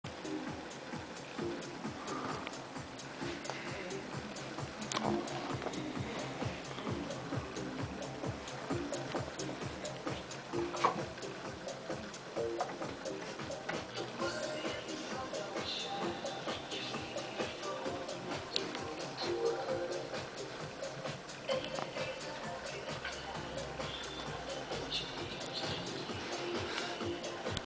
The Jumping Gerbil - Ninkin Poop Part 2...
This is the second part of Ninkin Poop where hes faced with a difficult challange to get back into his cage!
The Poopster would have to use his talent to jump up onto certain things to climb back into his cage.